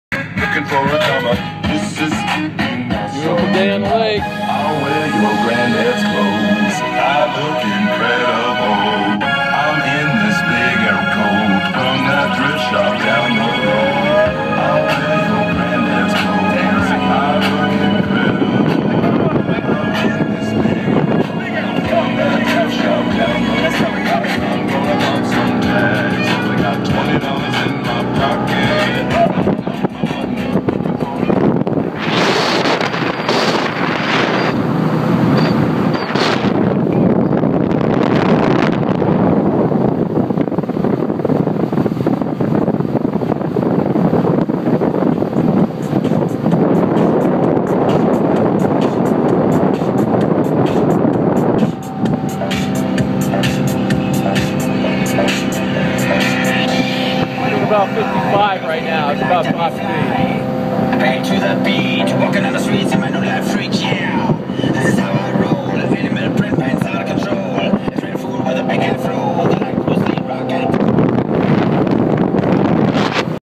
1983 BAJA TC190 getting up to speed on winnipesaukee